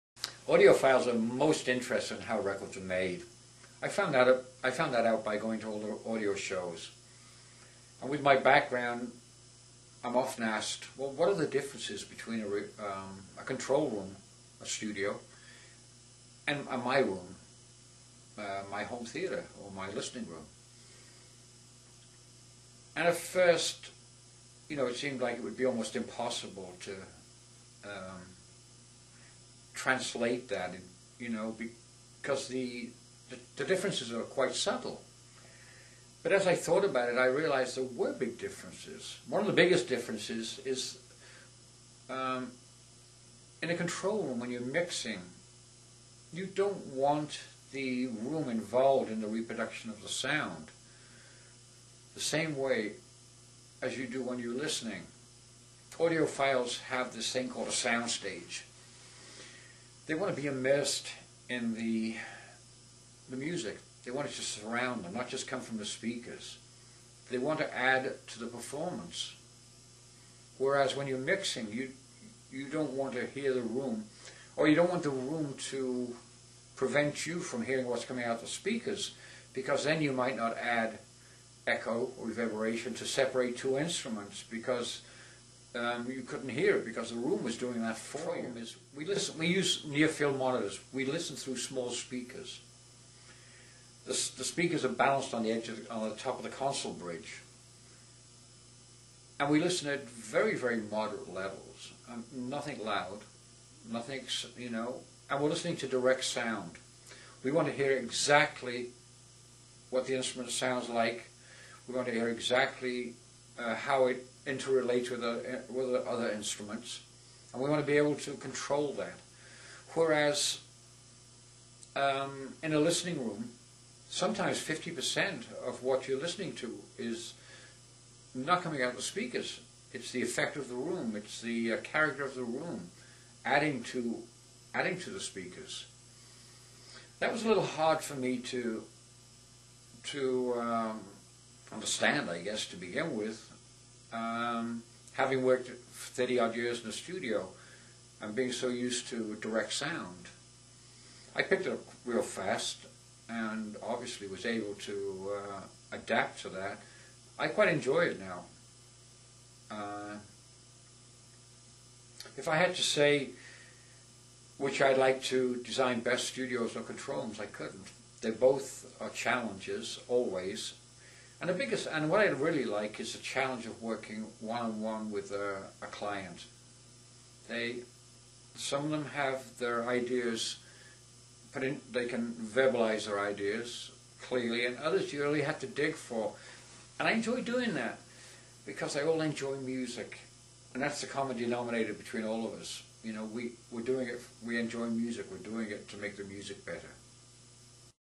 Listening room and Home theater design
Christopher Huston, Vice President, Acoustical Engineering for Rives Audio discusses some of the differences of studio versus home theater and listening room designs.